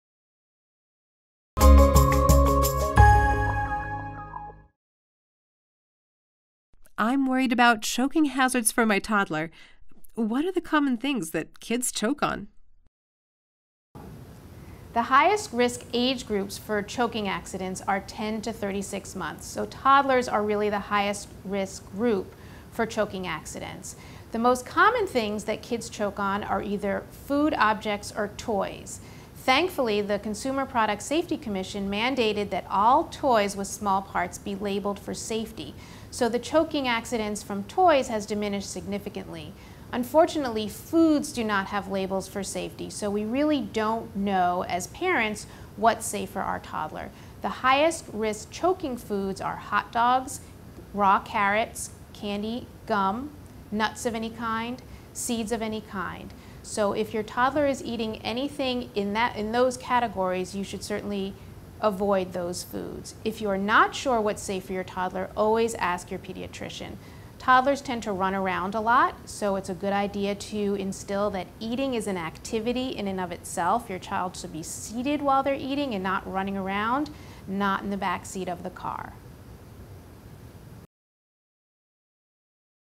What are the common things that kids choke on? | Choking Hazards - Nina Shapiro, MD
I'm worries about choking hazards for my toddler. What are the common things that kids choke on?